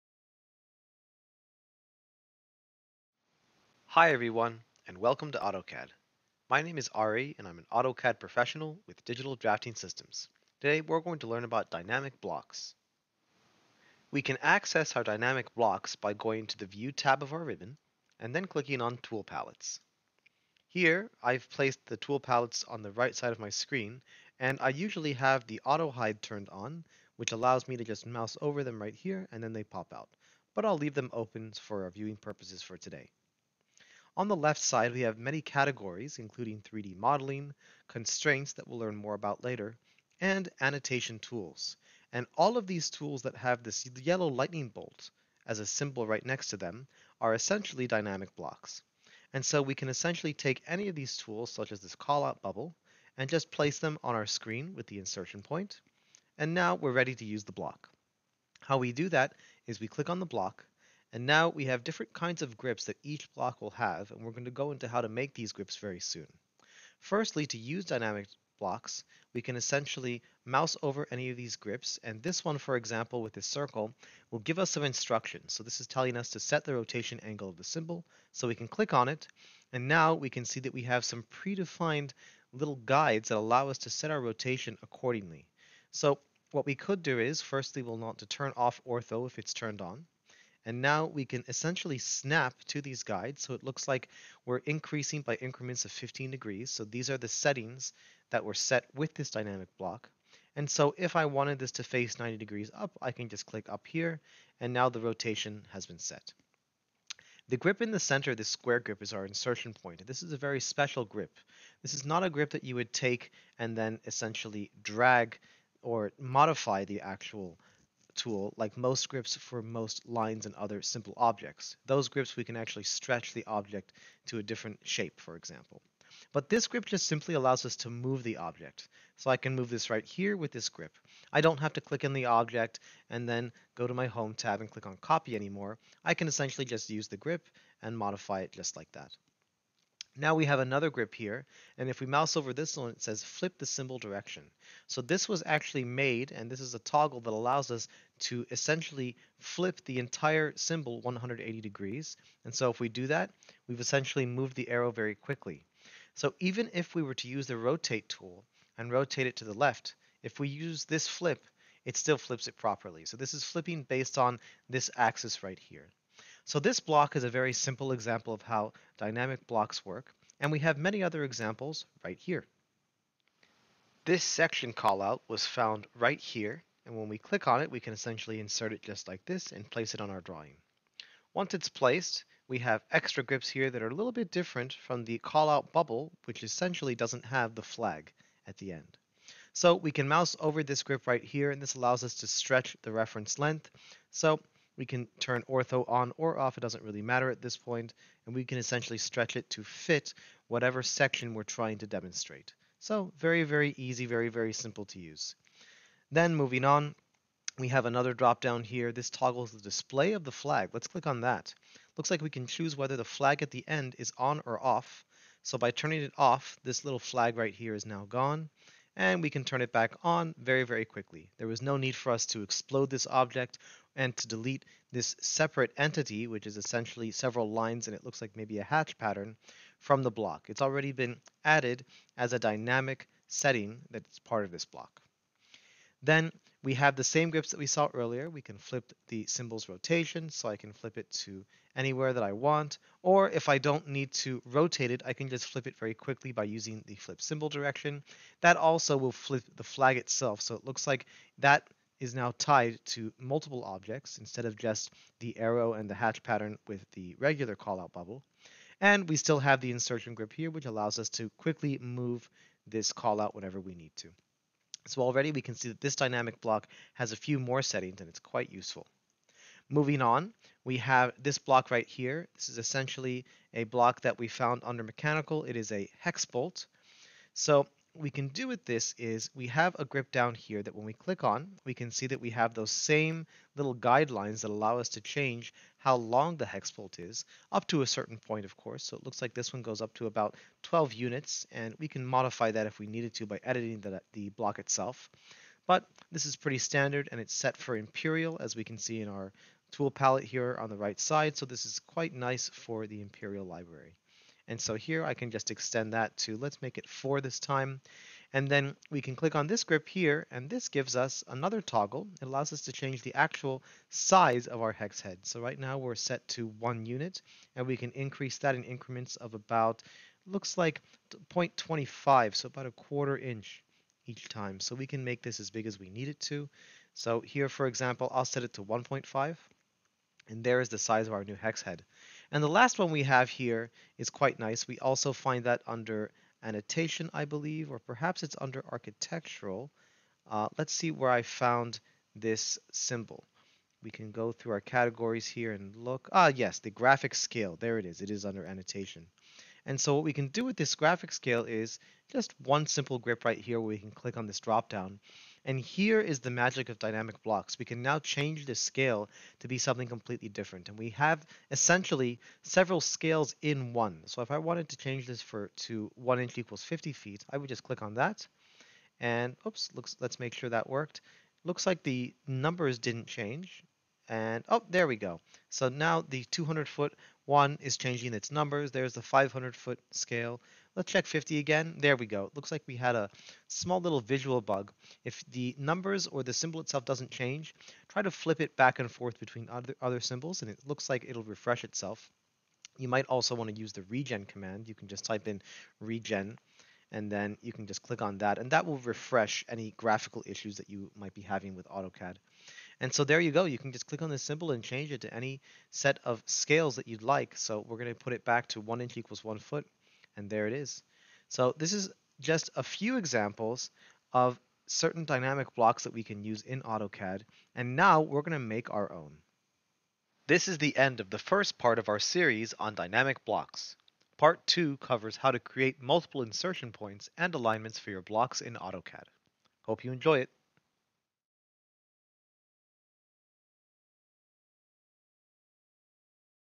Using Dynamic Blocks & The Tool Palette in AutoCAD - Part 1
Join our AEC Technical Specialist Ariel Rejtman as he shows us all about dynamic blocks in AutoCAD in this 5 part series. In part 1 we are introduced to dynamic blocks and the tool palette in AutoCAD.